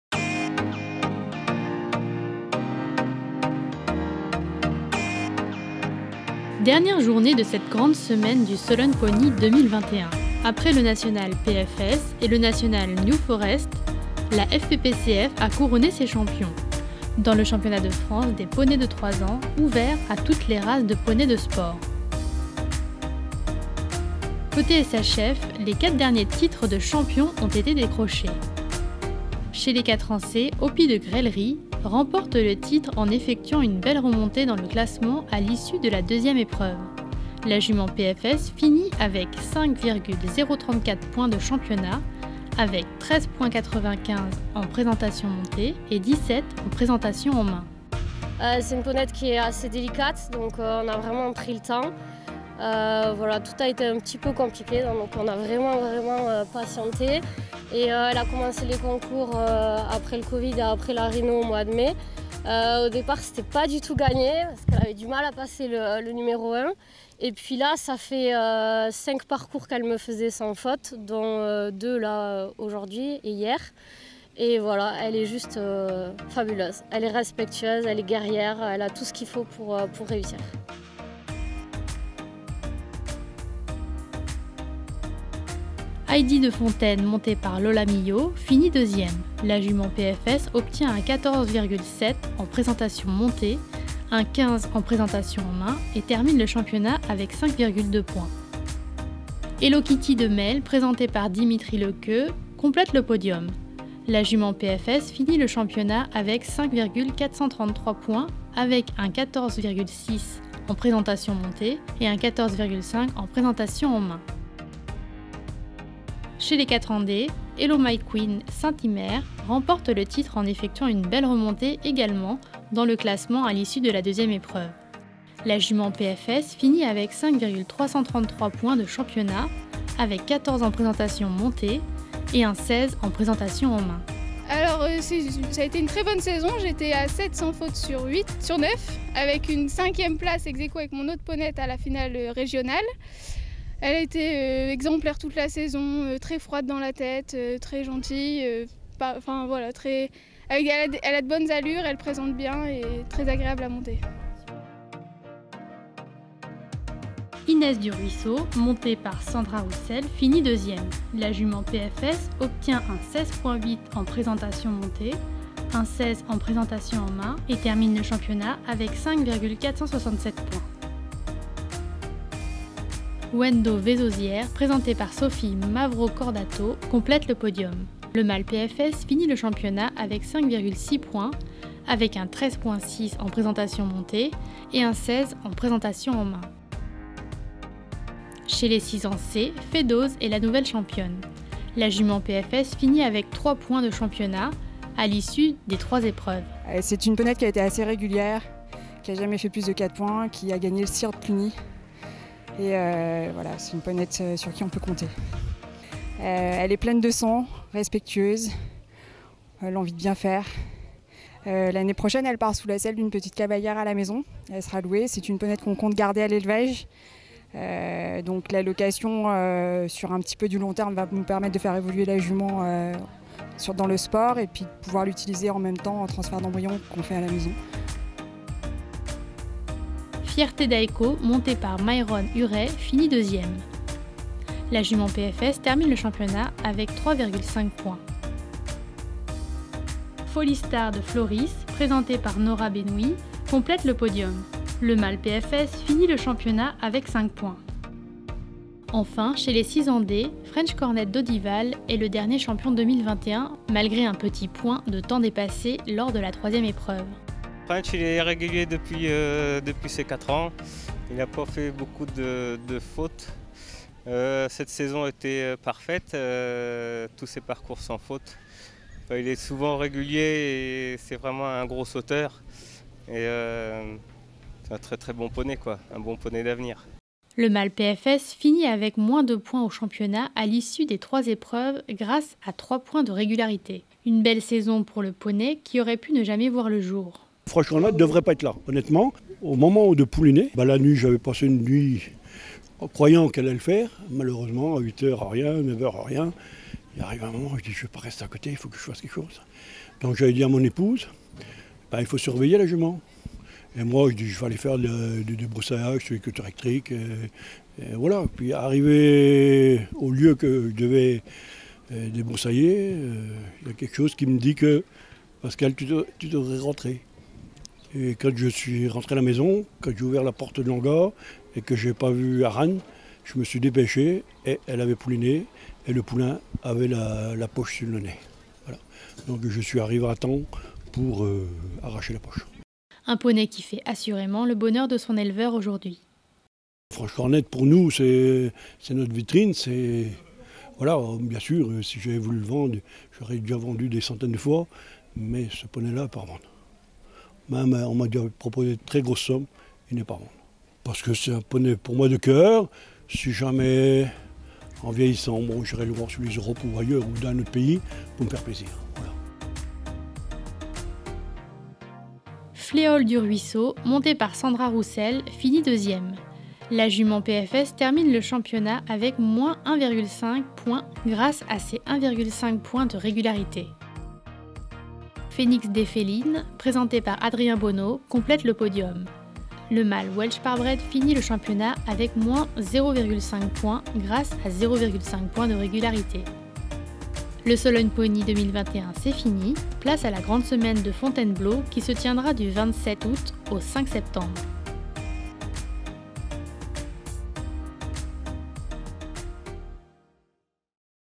Sologn'Pony 2021 : La Quotidienne de l'Elevage - Samedi 21 août
Clap de fin pour la 22ème édition du Sologn’Pony !
Tous les Champions 2021 ont été sacrés et c’est pour nous le temps de faire un point sur les nouveaux Champions des 4 et des 6 ans !

Toutes nos Félicitations aux concurrents, éleveurs et propriétaires et RDV en 2022 😃